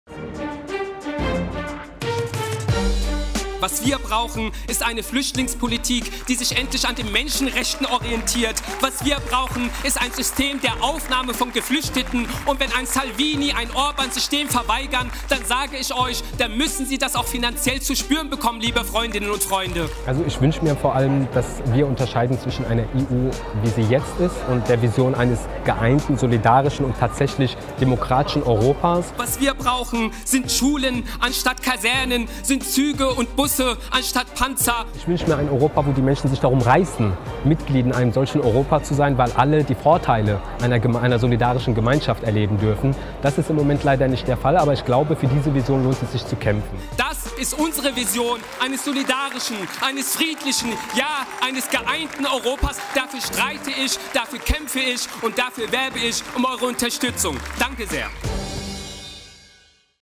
Ali Al-Dailami - Kandidat zur Europawahl 2019 stellt sich vor
Ali Al-Dailami kandidiert für den Listenplatz 6 zur Europawahl.